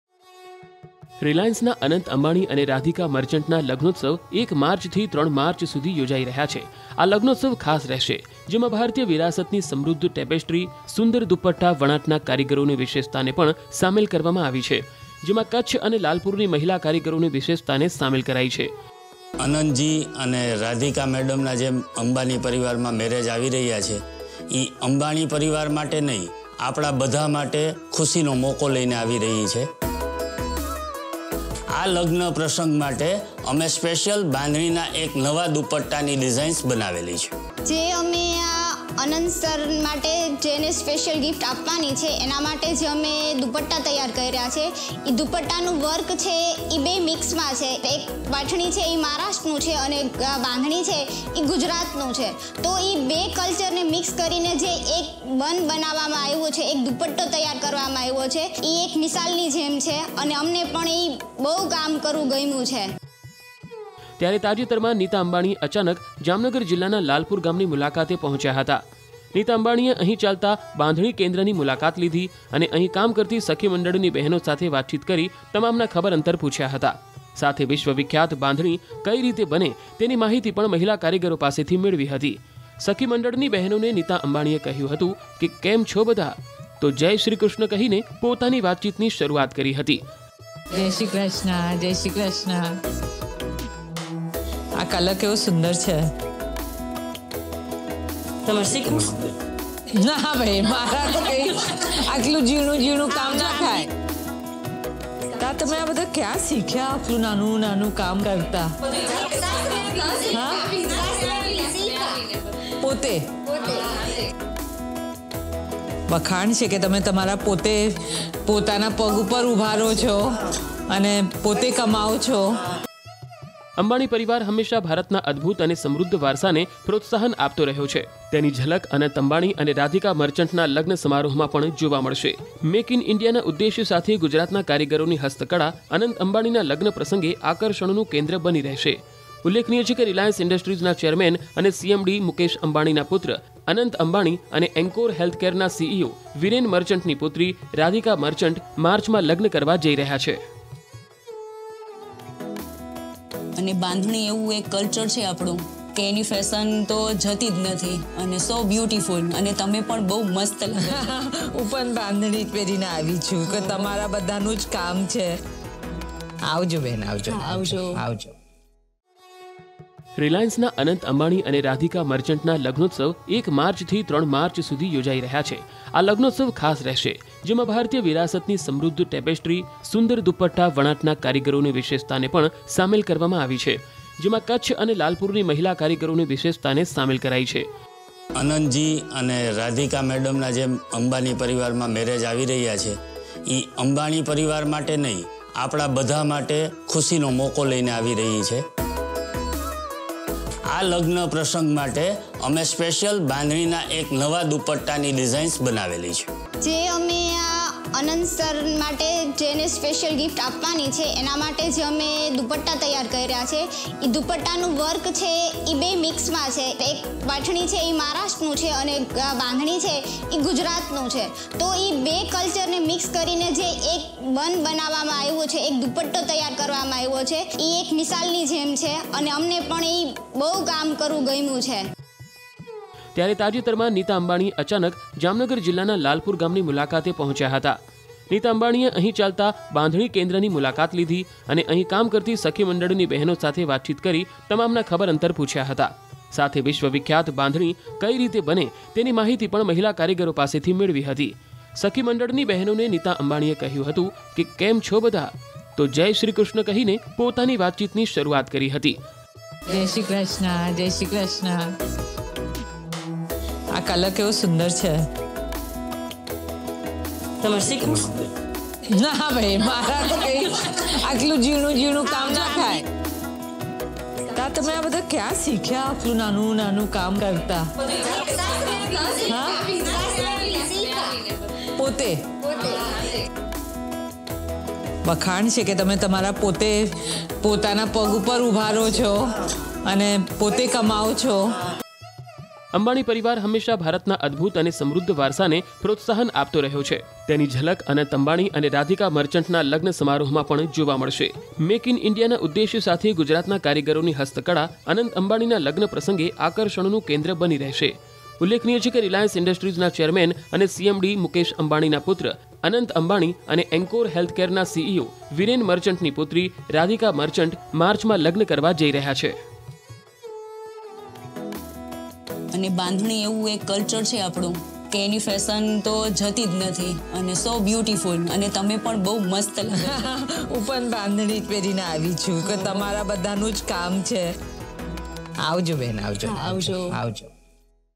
Jamnagar News : નીતા અંબાણી પહોંચ્યા કારીગરોને મળવા | Bandhani | Gujarati Samachar | News18 | N18V
Preparation for the wedding of Anant Ambani and Radhika Merchant. The women artisans of Kutch and Lalpur are preparing special dupatta. Nita Ambani arrived to meet the artisans. Talking to the women artisans saying Jai Shri Krishna.

અનંત અંબાણી અને રાધિકા મર્ચન્ટના લગ્નની તૈયારી.કચ્છ અને લાલપુરની મહિલા કારીગરો તૈયાર કરી રહ્યા છે ખાસ દુપટ્ટા.નીતા અંબાણી પહોંચ્યા કારીગરોને મળવા.જય શ્રી કૃષ્ણ કહીને મહિલા કારીગરો સાથે કરી વાતચીત. n18oc_jamngar